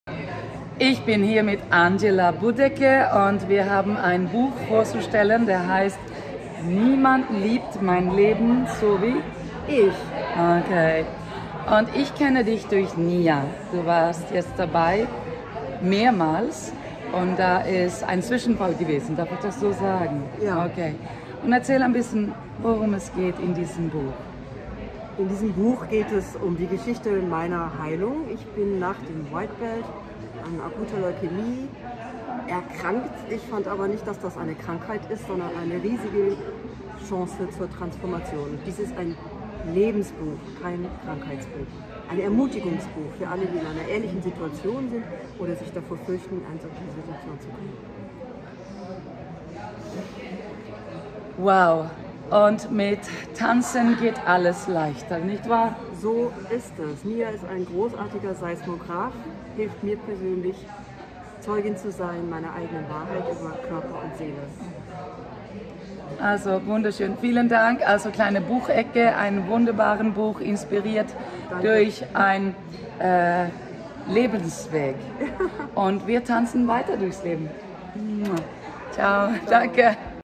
Interview mit Angela im Nia Brownbelt Training in Hamburg 2024
Angela hat ein Buch über ihren Heilungsweg geschrieben "Niemand liebt mein Leben so wie ich“ - und jetzt hat sie ihren Brownbelt gemacht!